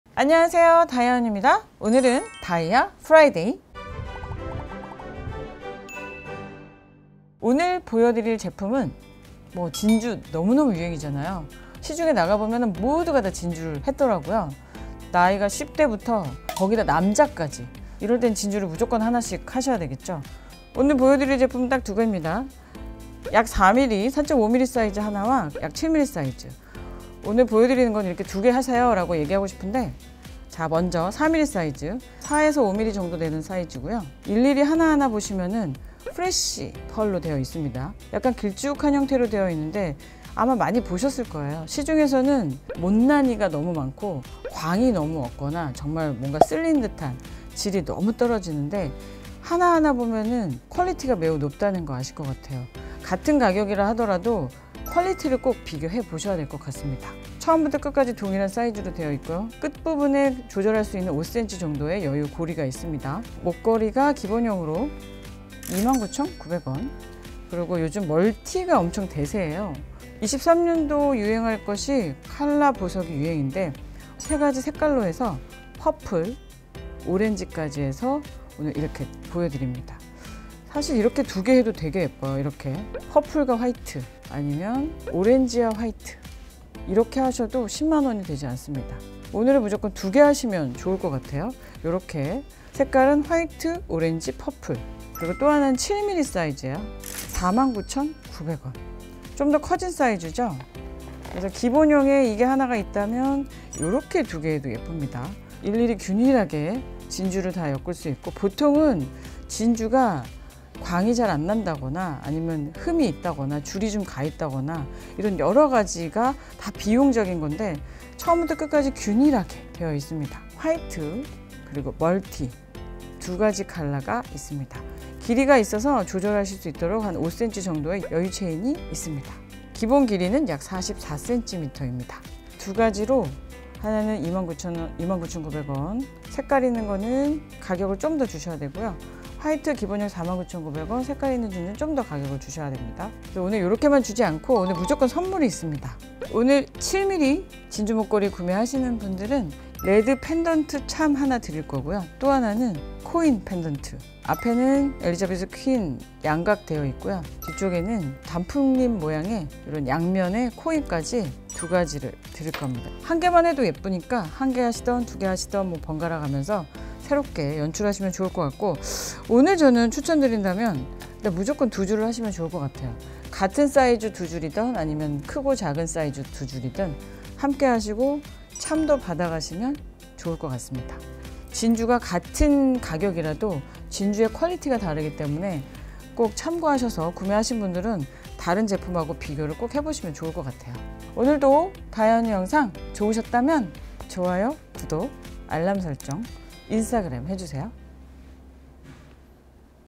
[공구] 요즘 대세! 트렌디한 진주 목걸이 2종😎 퀄리티 비교불가 가격부담없이 고르세요🤩_[다이아언니]
안녕하세요 다이아언니입니다 💎
오늘은 오벌 쉐입 진주 목걸이
2가지 사이즈로 준비했습니다 😘

퀄리티 감히 비교불가입니다~ㅎ
파격적인 가격으로 준비했으니
구독자분들에게 행복을 주는 공구 되길 바라요 🤩

즐거운 주말 보내시고 다른 콘텐츠도 많이 사랑해주세요~

다이아언니 영상은 매주 화요일 에 업로드가 됩니다^^👋
다이아 프라이데이는 금요일 18:30분~